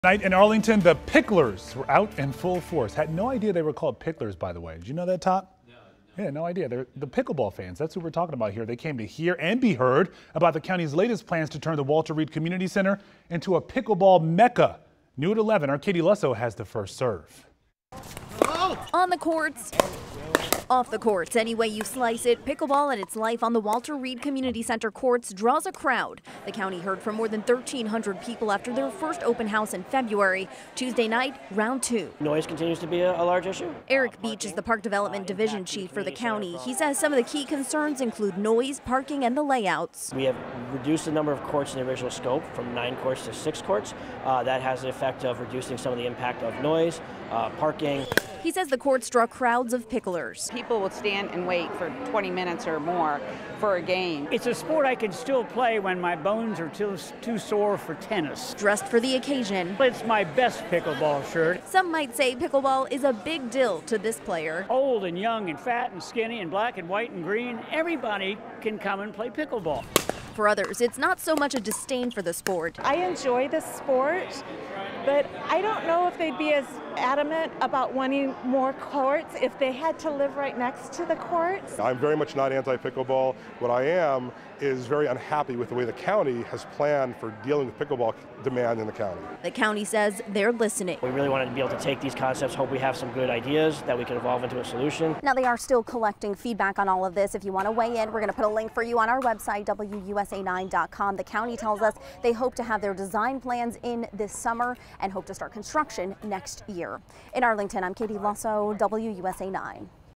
Open house on pickleball plans in Arlington
The county heard from more than 1,300 people after their first open house in February. Tuesday night was round two.